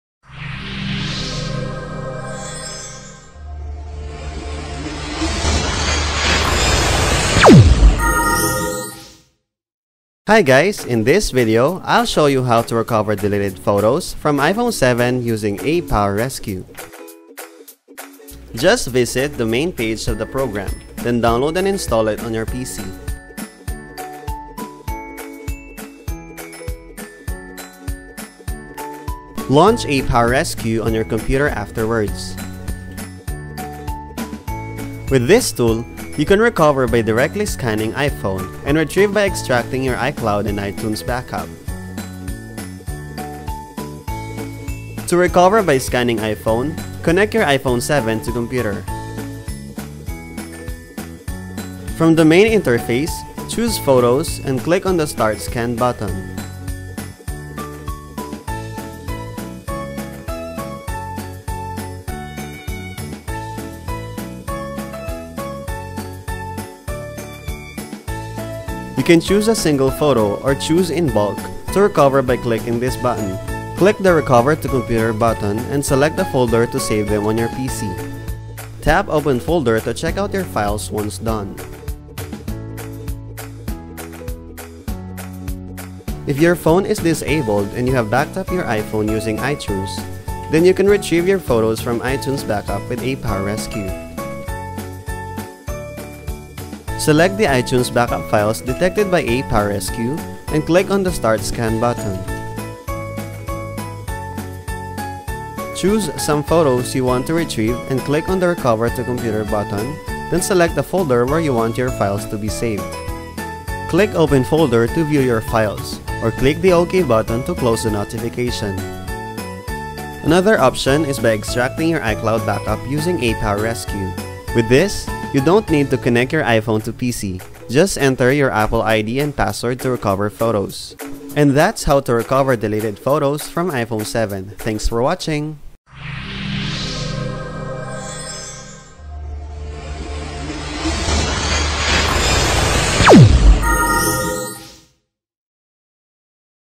How to Recover Deleted Photos from iPhone 7
If you want to recover deleted photos from iPhone 7, ApowerRescue is perfect for you. Check this video to learn the detailed steps.